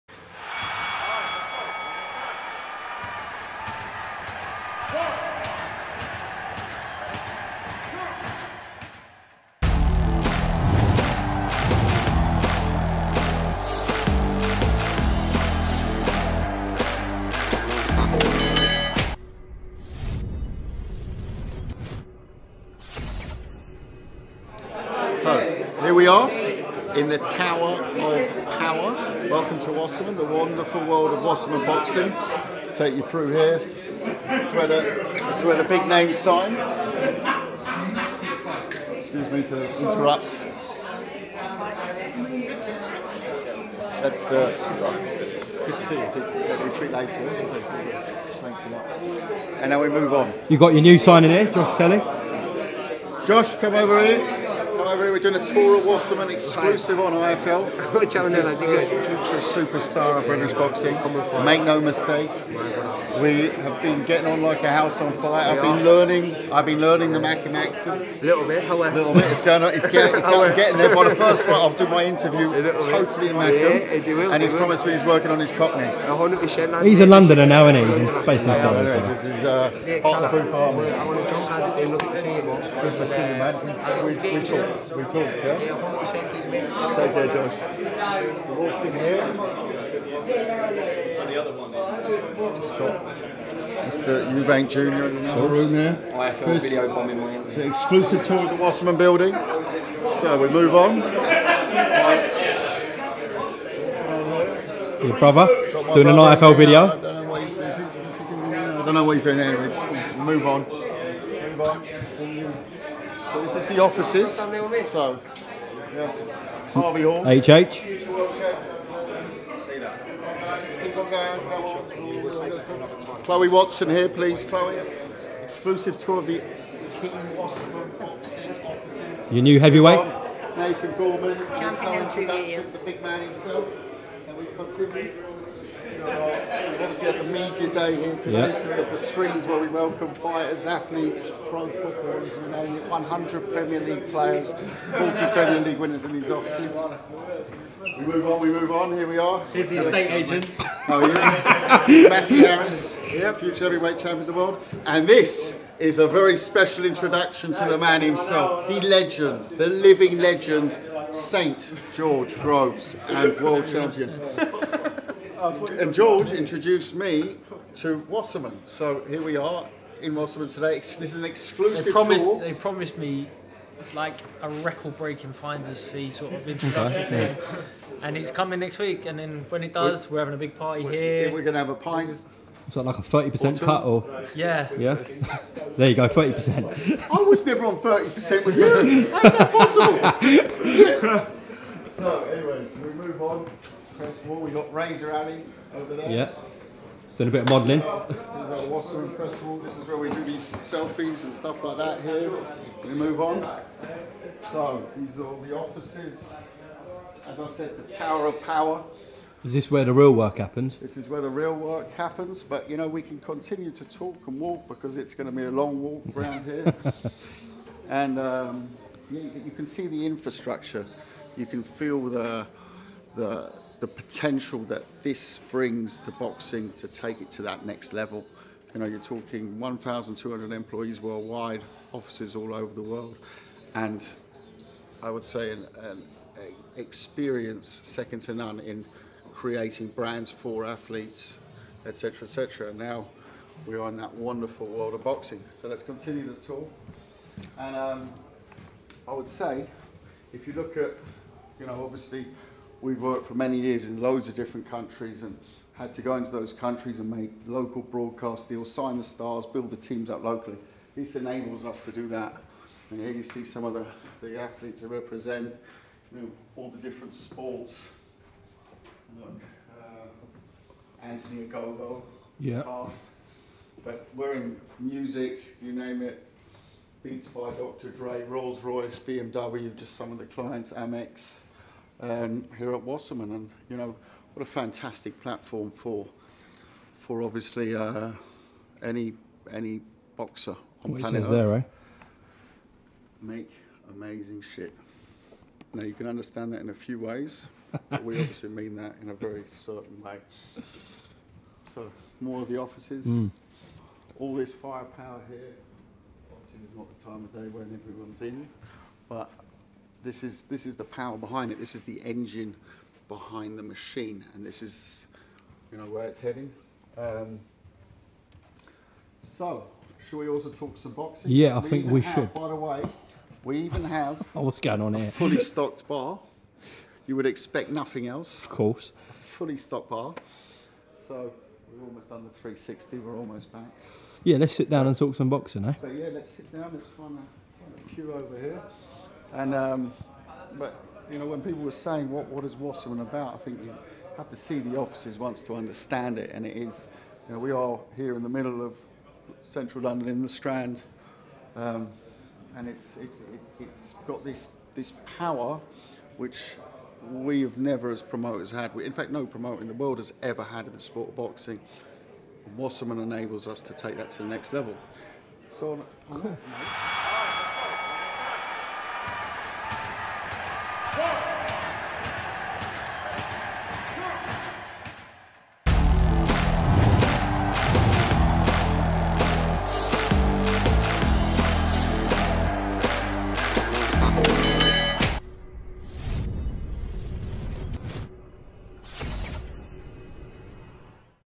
DON KALLE SAUERLAND GIVES A TOUR OF WASSERMAN HQ (INCLUDING BAR AREA!) - WITH GROVES, KELLY & GORMAN
Proud supporters of Fight Picks, boxing’s only FREE TO PLAY pick ‘em game - pick your predictions to play and win cash prizes.

*VIDEO BY @UMAR_IFL*